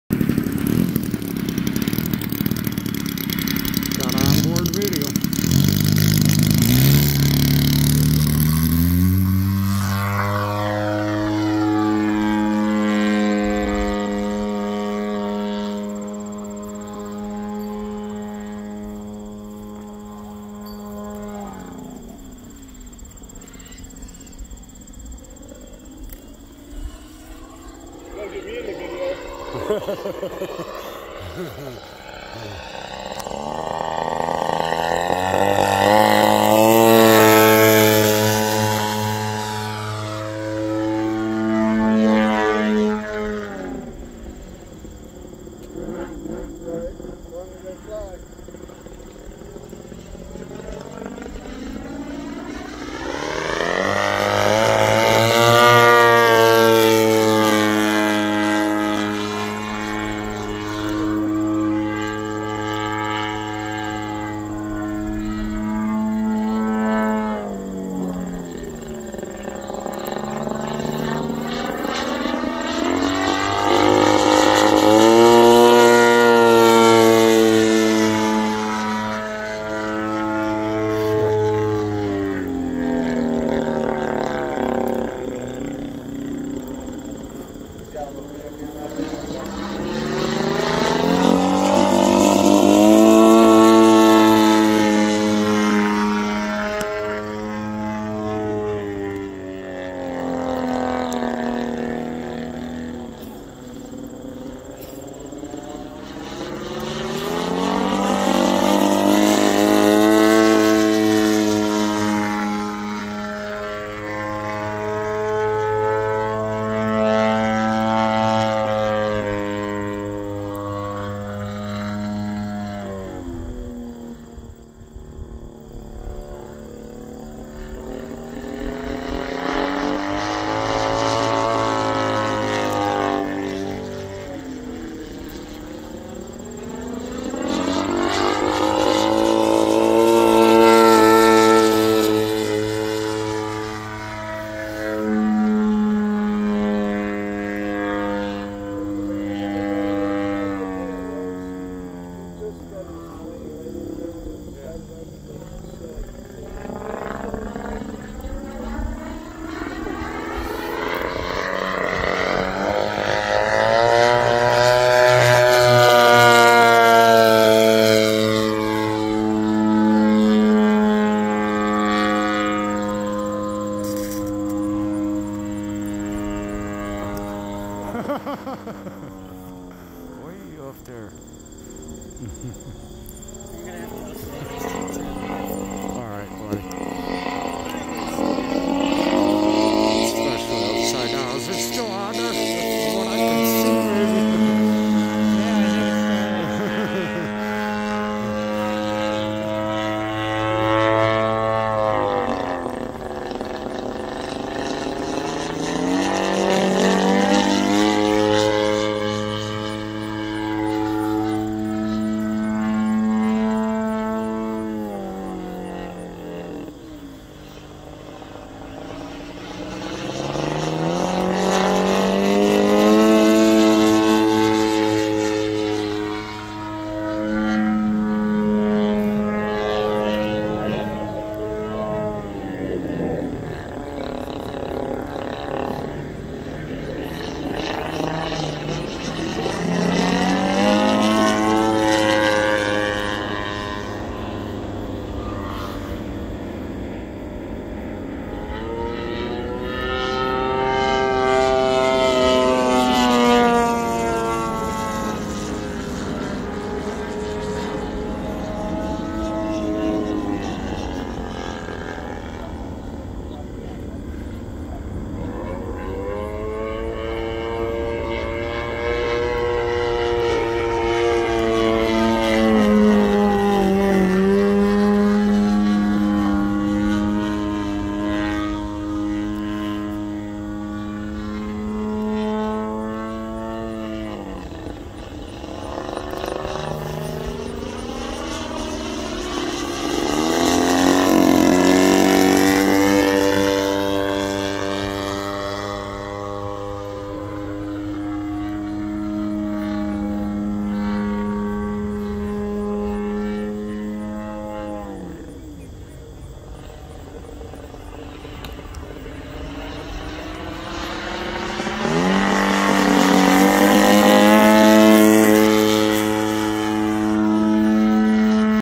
Pilot Extra 330 sc 107 in.wing Gp123 twin
Scott's Pilot Extra 330 SC 107 inch wingspan.  GP123  twin , Falcon 29x9 carbon prop.   SPEKTRUM powersafe 12 channel receiver.  MKS 777a+ on the tail, MKS 777 on the Ailerons.  Two 3500 lipo receiver batteries